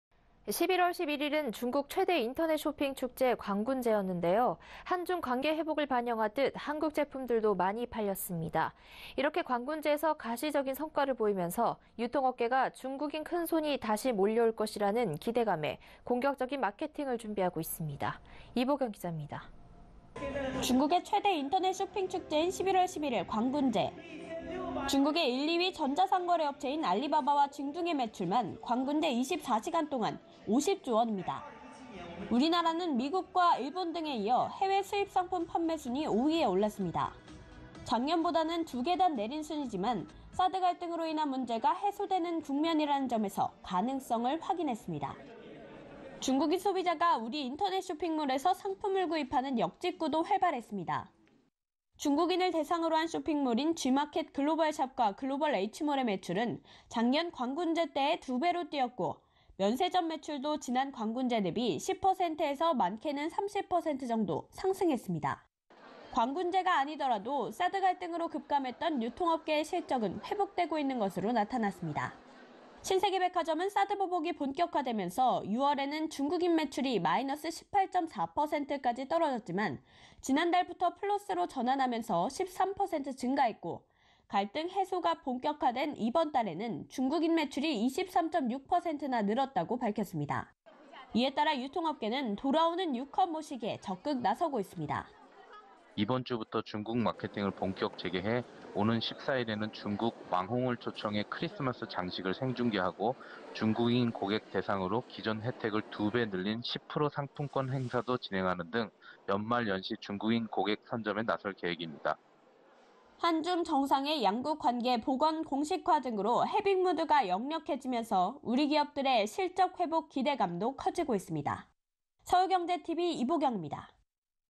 [서울경제TV] 광군제 판매 순위 5위… “돌아오는 유커 잡아라”
[앵커]
11월11일은 중국 최대 인터넷 쇼핑축제인 광군제였는데요. 한중 관계회복을 반영하듯 한국 제품들도 많이 팔렸는데요. 이렇게 광군제에서 가시적인 성과를 보이면서, 유통업계는 중국인 큰손이 다시 몰려올 것이라는 기대감에 공격적인 마케팅을 준비하고 있습니다. 이보경기잡니다.

[기자]
중국의 최대 인터넷 쇼핑 축제인 11월11일 광군제.
중국의 1,2위 전자상거래 업체인 알리바바와 징둥의 매출만 광군제 24시간동안 50조원입니다.
우리나라는 미국과 일본 등에 이어 해외수입상품 판매순위 5위에 올랐습니다.
작년보다는 두계단 내린 순위이지만 사드 갈등으로 인한 문제가 해소되는 국면이라는 점에서 가능성을 확인했습니다.
중국 소비자가 우리 인터넷 쇼핑몰에서 상품을 구입하는 ‘역직구’도 활발했습니다.
중국인을 대상으로한 쇼핑몰인 G마켓 글로벌샵과 글로벌 H몰의 매출은 작년 광군제 때의 두배로 뛰었고, 면세점 매출도 지난 광군제 대비 10%에서 많게는 30% 정도 상승했습니다.
광군제가 아니더라도, 사드갈등으로 급감했던 유통업계 실적은 회복되고 있는 것으로 나타났습니다.
신세계백화점은 사드 보복이 본격화되면서 6월에는 중국인 매출이 -18.4%까지 떨어졌지만 지난달부터 플러스로 전환하면서 13% 증가했고, 갈등 해소가 본격화된 이번 달에는 중국인 매출이 23.6%나 늘었다고 밝혔습니다.
이에 따라 유통업계는 돌아오는 유커 모시기에 적극 나서고 있습니다.

[인터뷰] 김종민 / 신세계백화점 홍보팀 과장
이번주부터 중국 마케팅을 본격 재개해, 오는 14일에는 중국 왕홍을 초청해 크리스마스장식을 생중계하고 중국인 고객을 대상으로 기존혜택을 두배 늘린 10% 상품권행사도 진행하는 등 연말연시 중국인 고객 선점에 나설 계획입니다.

한중정상의 양국관계 복원 공식화 등으로 해빙무드가 역력해지면서 우리 기업들의 실적회복 기대감도 커지고 있습니다.

[영상편집 김지현]